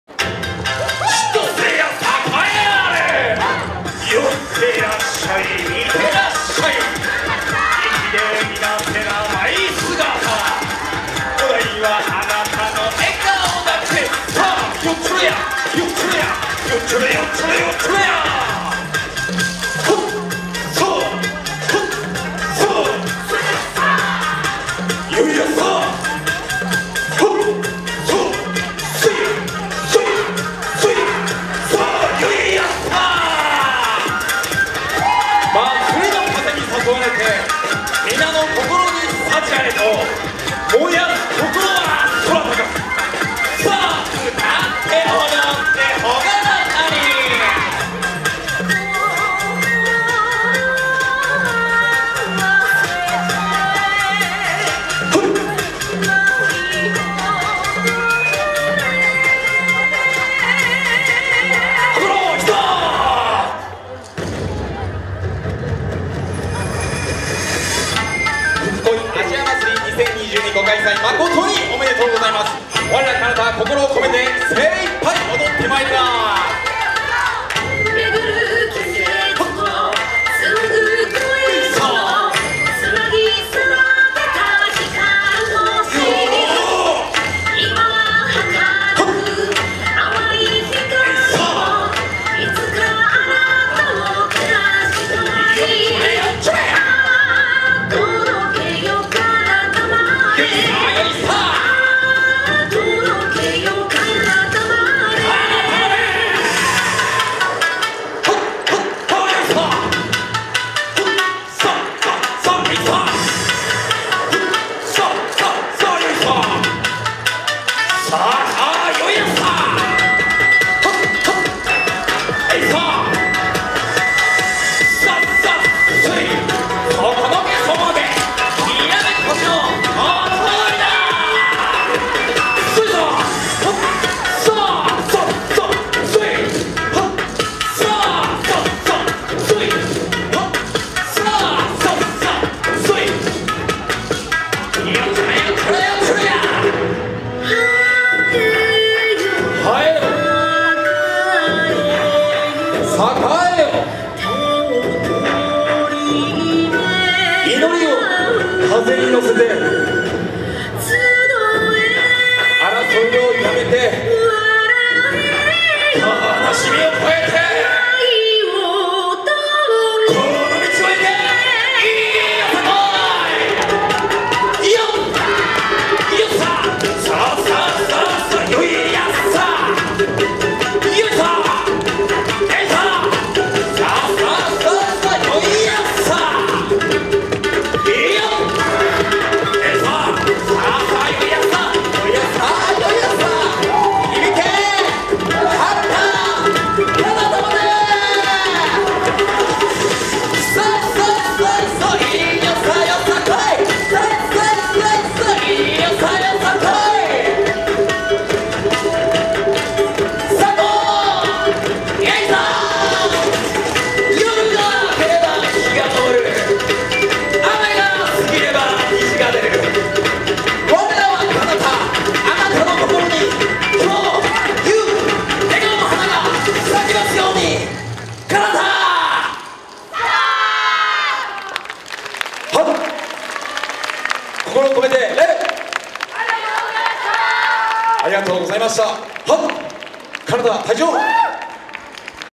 【煌星】ふくこいアジア祭り2022
ふくこいアジア祭り2022【煌星】さんの演舞です。【画質up版】